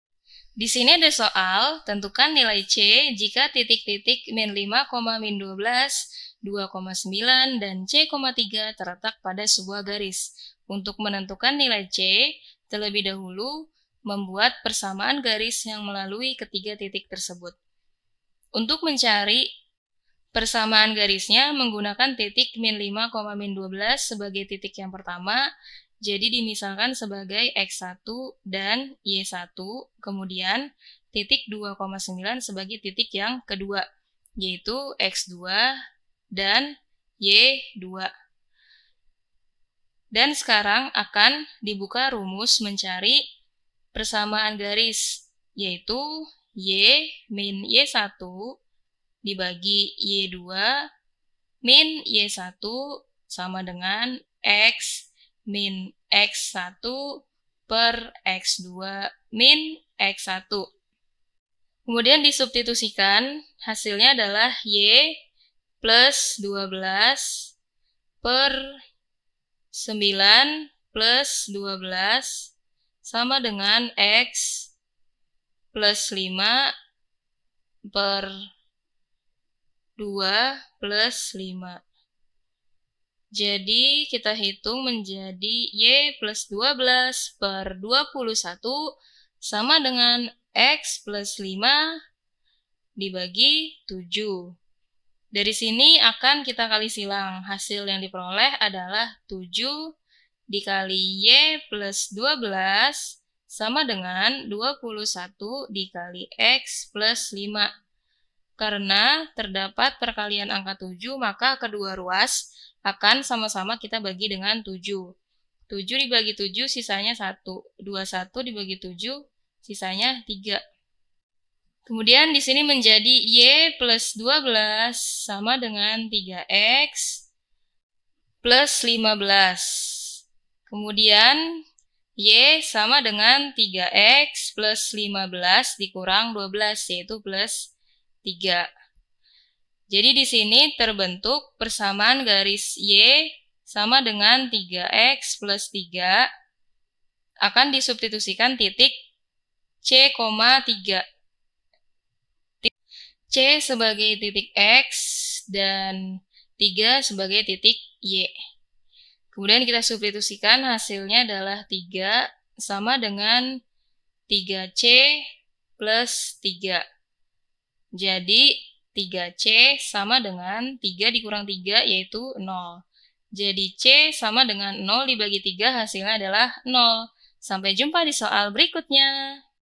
nilai C, jika Tentukan titik-titik (-5, -12), (2, 9), dan (C, 3) terletak pada sebuah garis.
Topik atau Materi: Persamaan Garis Lurus - PERSAMAAN GARIS LURUS - ALJABAR - Matematika Kelas 8 SMP - K-2006

00:00 : Bedah Soal
01:00 : Konsep, rumus dan pengertian Persamaan Garis Lurus
01:18 : Langkah penyelesaian soal
01:59 : Jawaban akhir
02:29 : Penutup